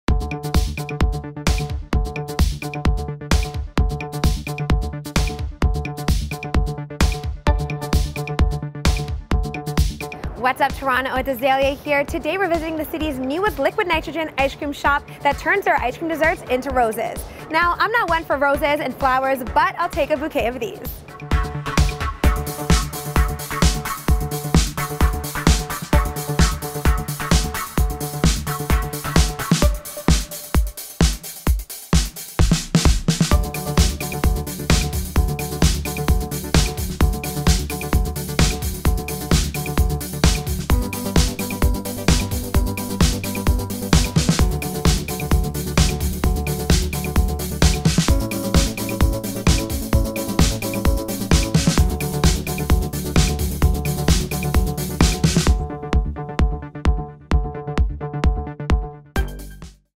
Cauldron Ice Cream in Toronto is does epic liquid nitrogen ice cream
Cauldron Ice Cream is a California-based chain of ice cream parlours, making ice cream to order in inventive rotating flavours using liquid nitrogen. Find them at 502 Queen St W.